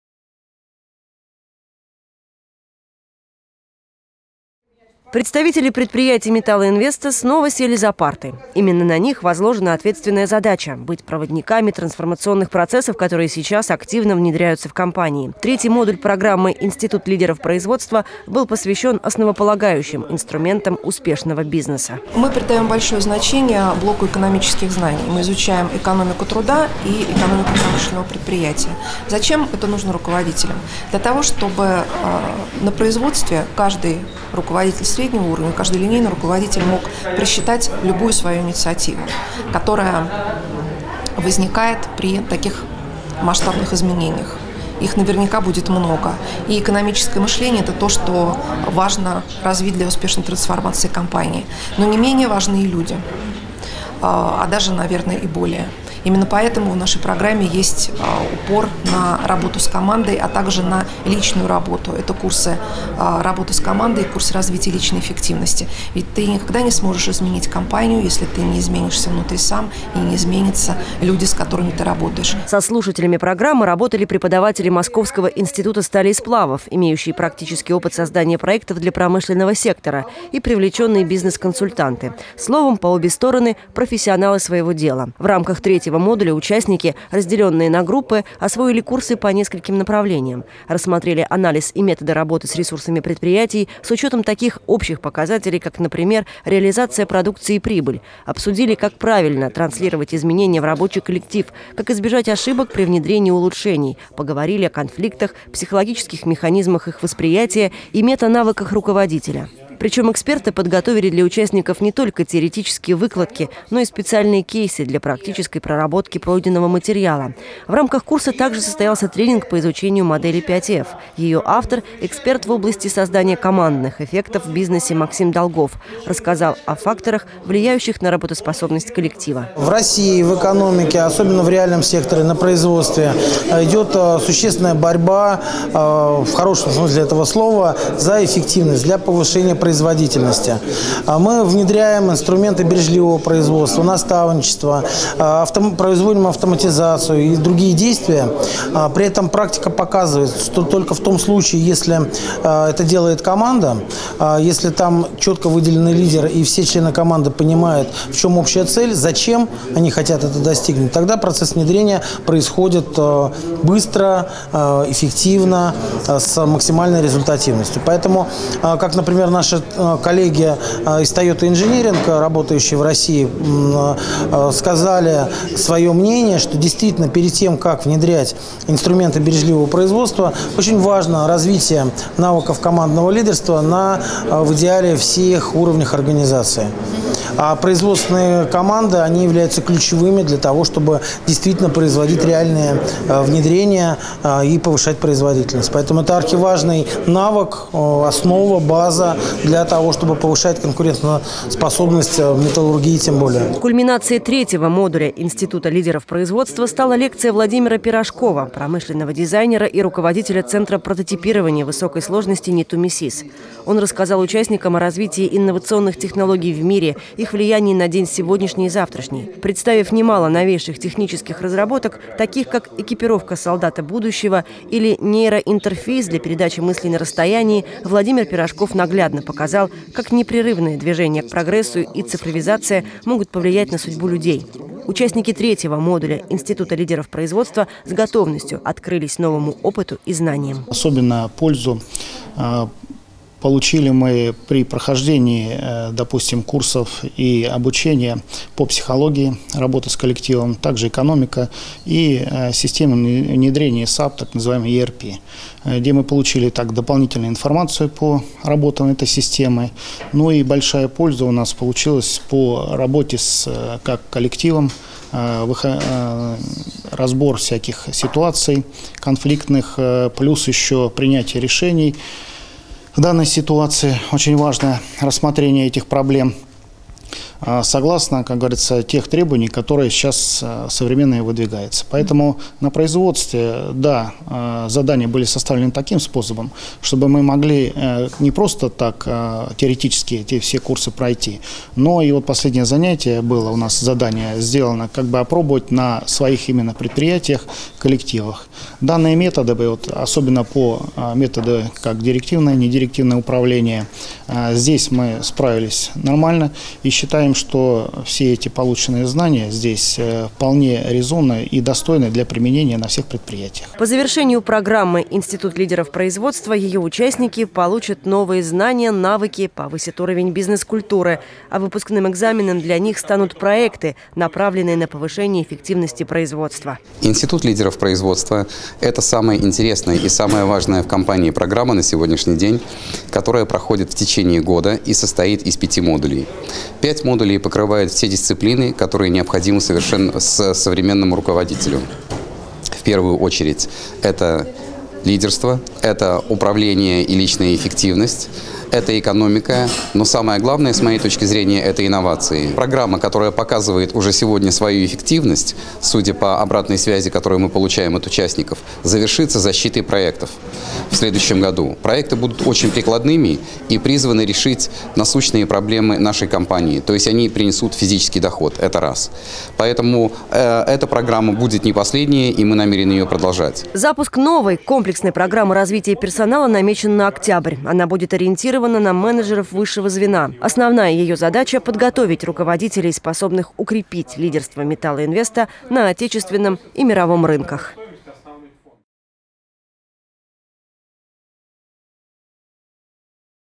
Корпоративная программа Металлоинвеста «Институт лидеров производства»
Курс третьего модуля прошли 146 руководителей среднего звена Лебединского и Михайловского ГОКов, ОЭМК и управляющей компании. В него вошел блок экономических дисциплин, а также тренинги по развитию личной эффективности и управлению изменениями. Занятия прошли на базе Старо Оскольского технологического института «МИСиС».